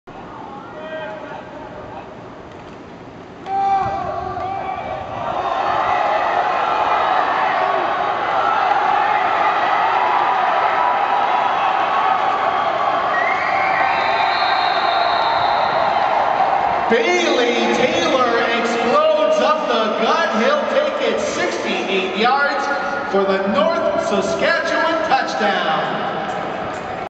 Taylor 67 yard TD run
Red River Cup 2013 Game #9. Saskatchewan North vs. Saskatchewan South.